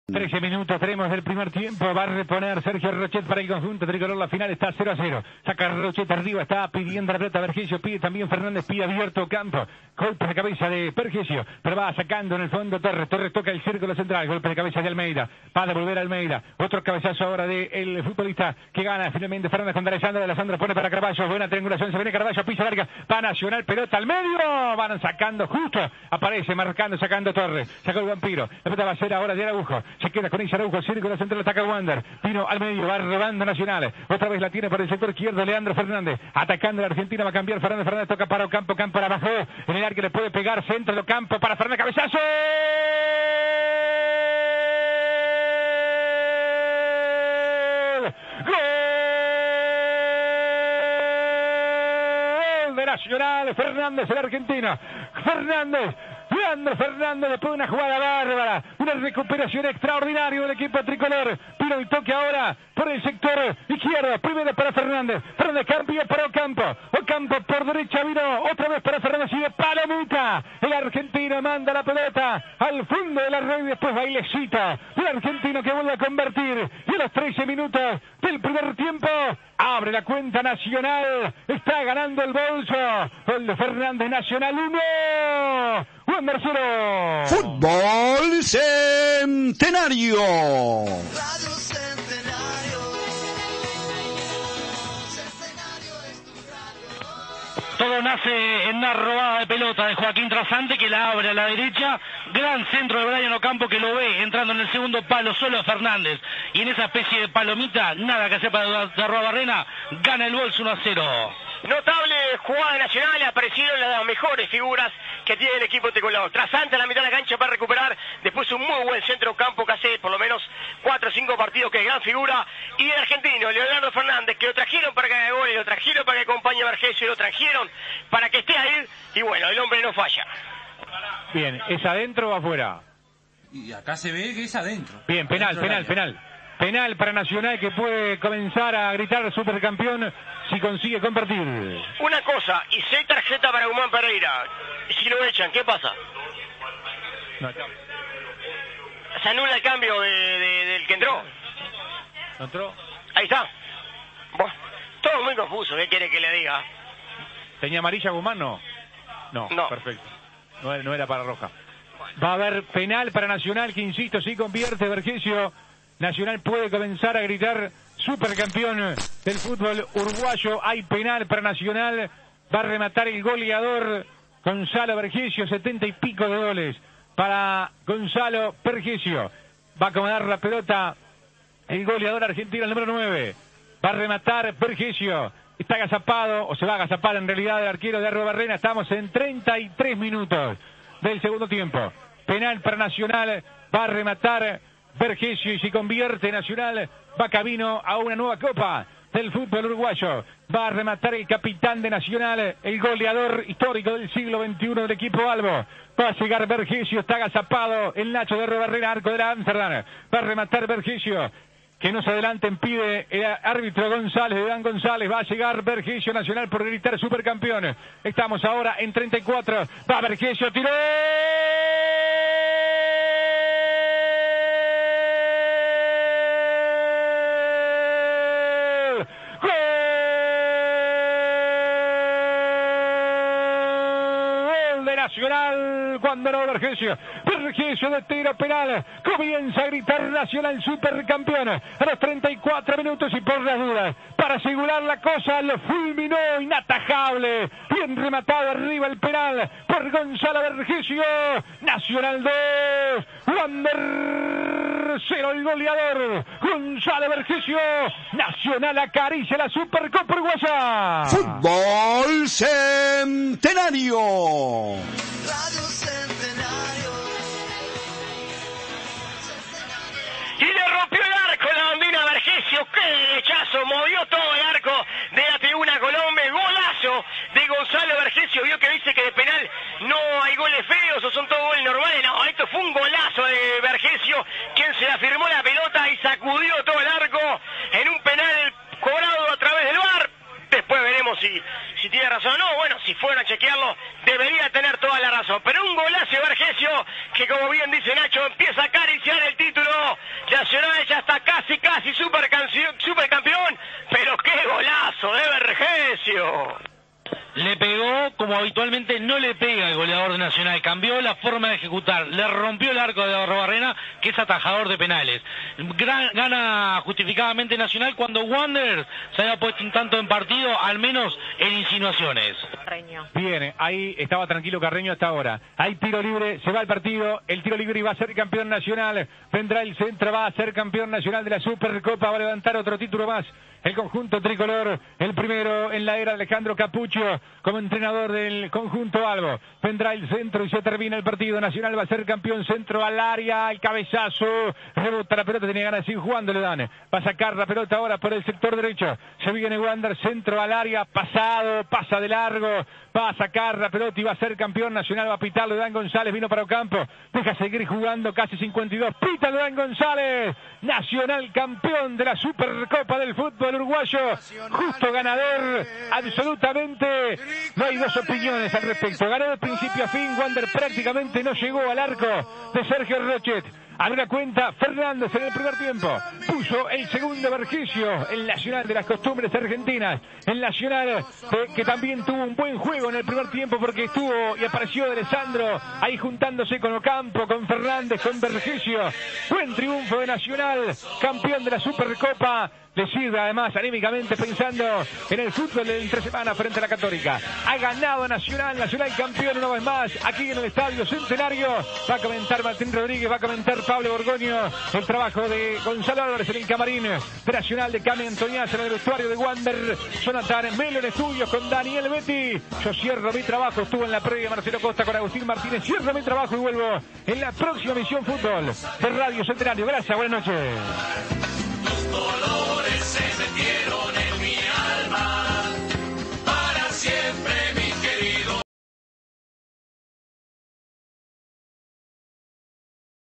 Nacional Campeón Supercopa 2021 (por Ignacio Saldombide)
Los goles y el final del partido, por Ignacio Saldombide para CX36 Radio Centenario 1250 AM.